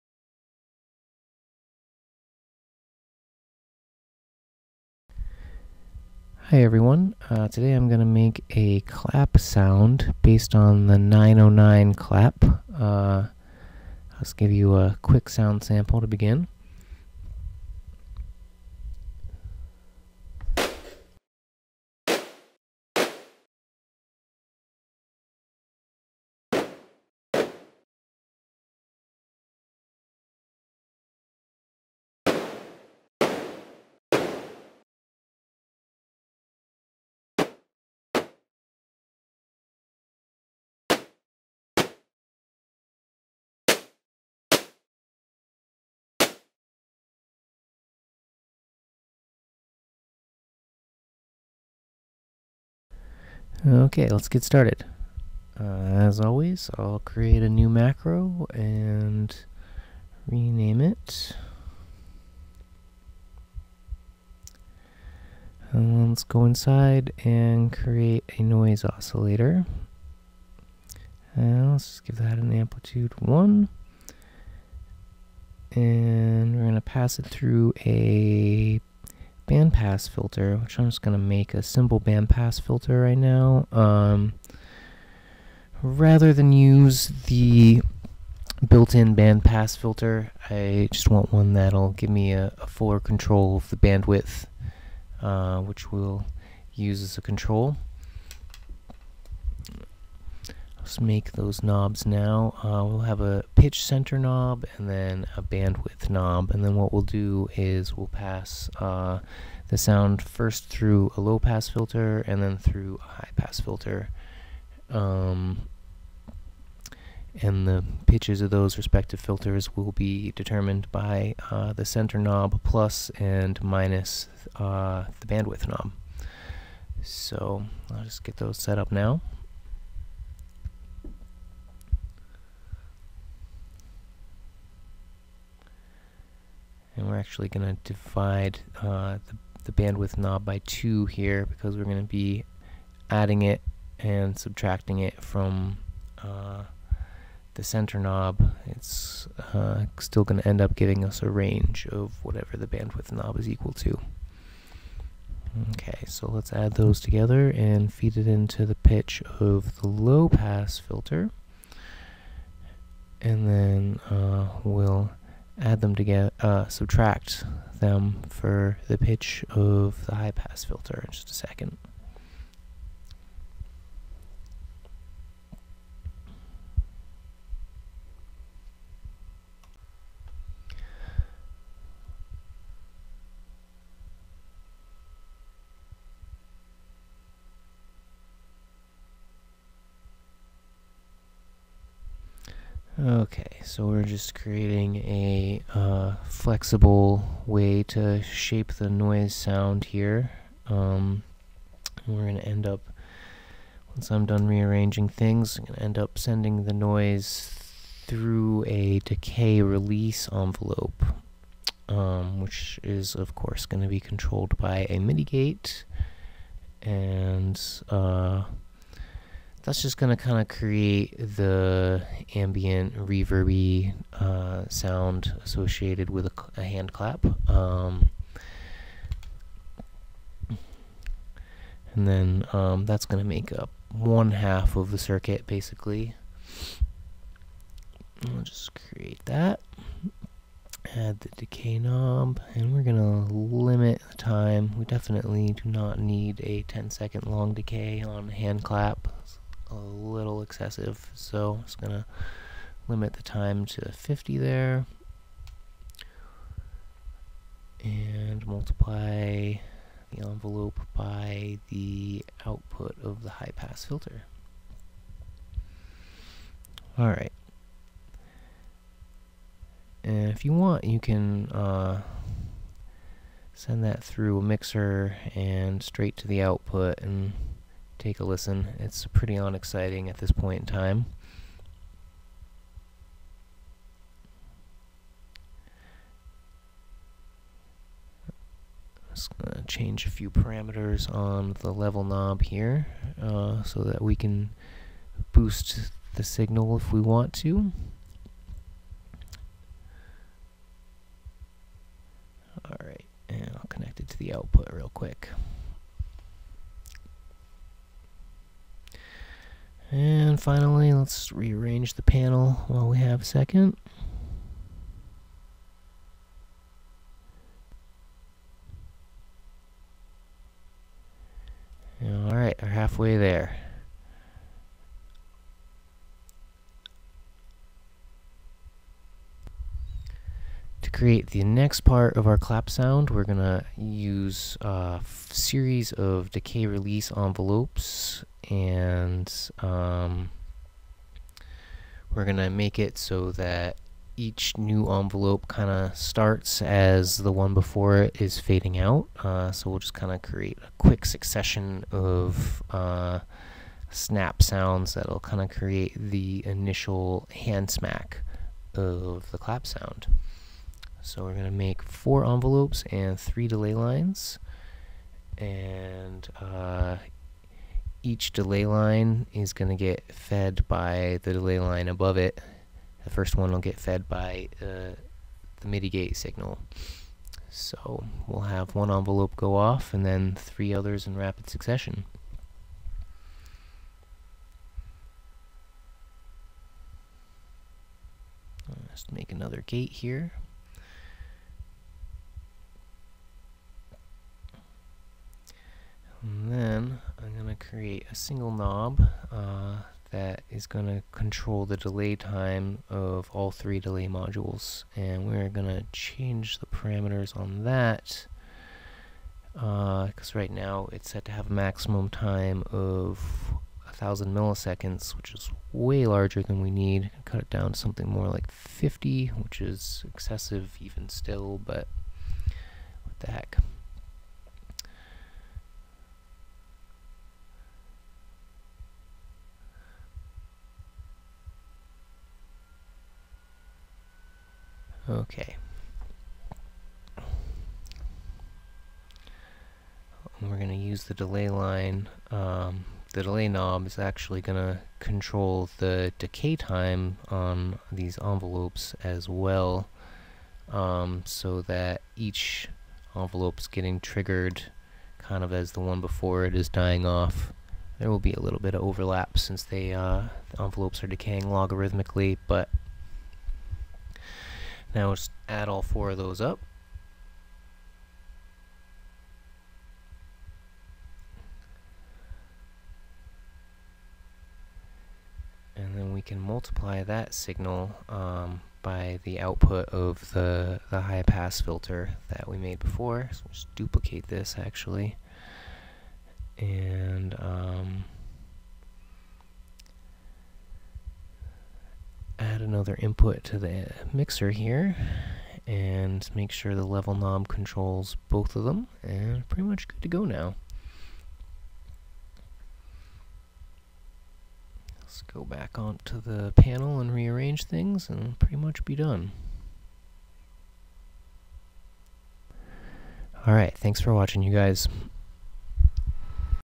Designing a 909 Clap in Reaktor
How to recreate a 909 style clap in Native Instruments Reaktor.

Starting out with a simple noise oscillator, which is routed through a bandpass filter module, this sound quickly develops. Some time is spent on getting the right knobs in place and adjusting their sensitivity to help ensure the right action for later tweaking. Once all of this has been set up the sound is passed through a decay/release envelope. This will provide the reverb and presence that this classic clap sound is known for. All of this can then be sent through a mixer and to the main output. After you arrange the knobs on your panel view, you can begin building the second half of this 909 clap instrument.

A series of decay/release envelopes are introduced now that will also help control the delay effect for this 909 clap. It's important to maintain the same control for all of the delay units, as well as the decay parameter of the envelopes, to ensure even and consistent delivery of the effect. Now all you need to do is rearrange the panel view so the new knobs are accessible and you can begin using this instrument!